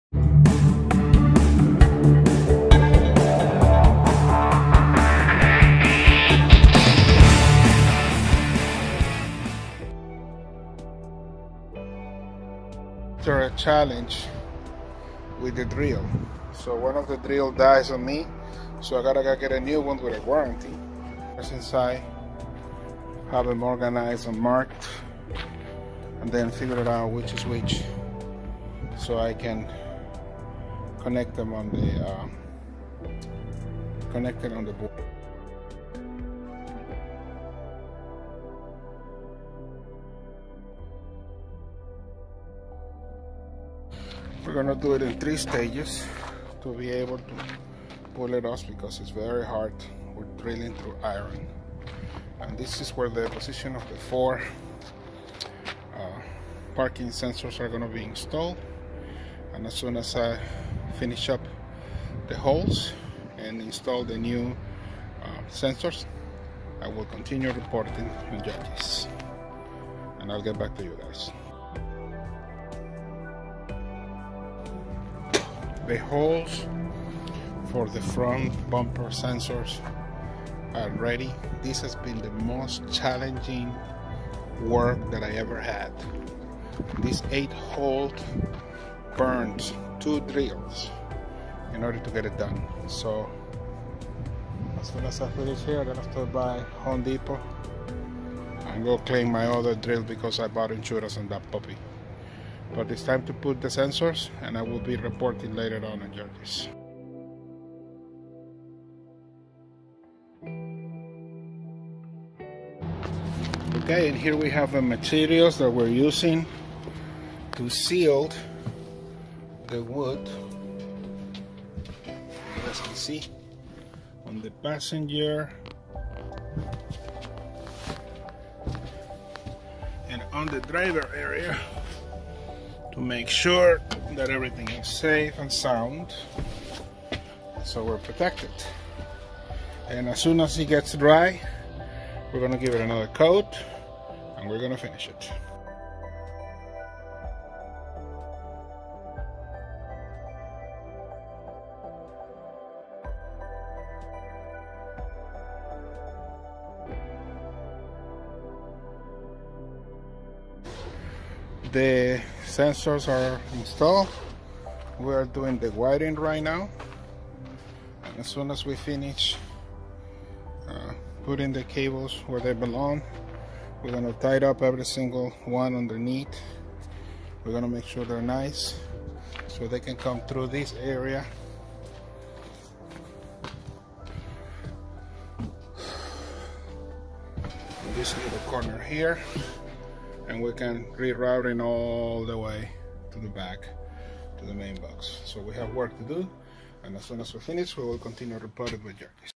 Installing new front and back parking sensors, GMC Motorhome the Yaryis adventures part 121
Today we are drilling the front and back bumpers burning two drills in the process, this was harder than I expected, also, we are repairing the wood floors on the driver and passenger sides, adding the wiring is a must and we are getting it done.

Follow our Yaryis Adventures